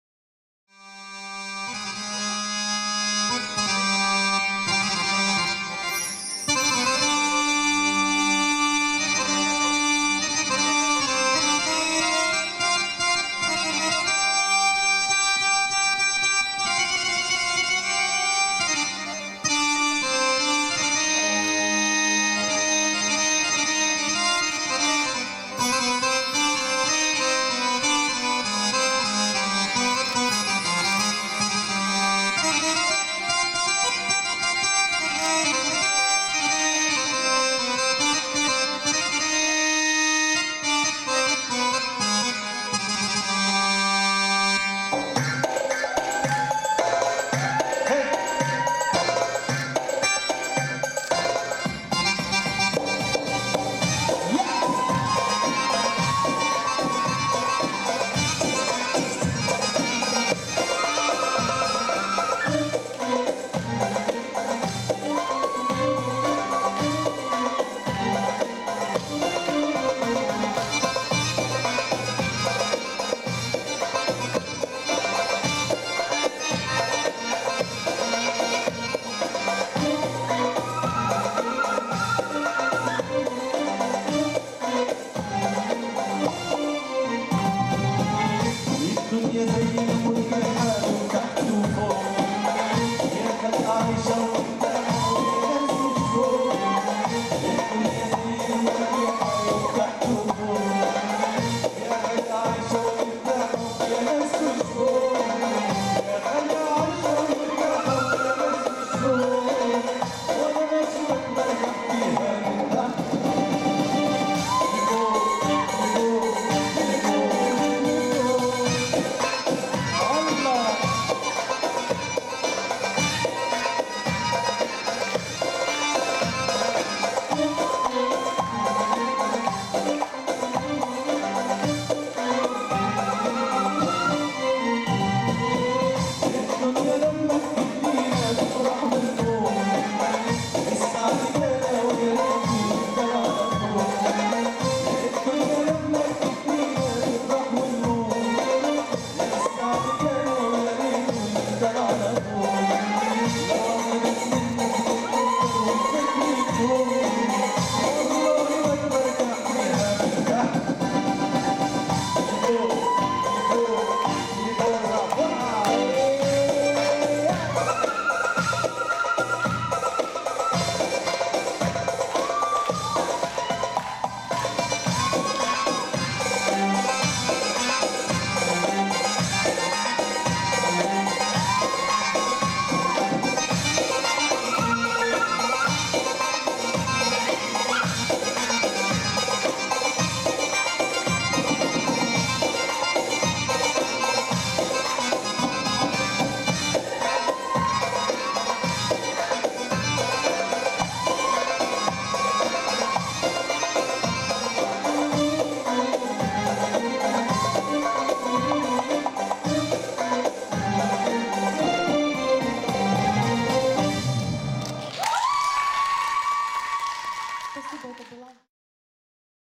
Maria Malinina ⊰⊱ Primorye CUP '14.
Мария Малинина на VI Кубке Приморья «Экспрессия Востока» г. Бердянск, гДК 03.08.14.
(Фольклор взрослые продолжающие)
 8,84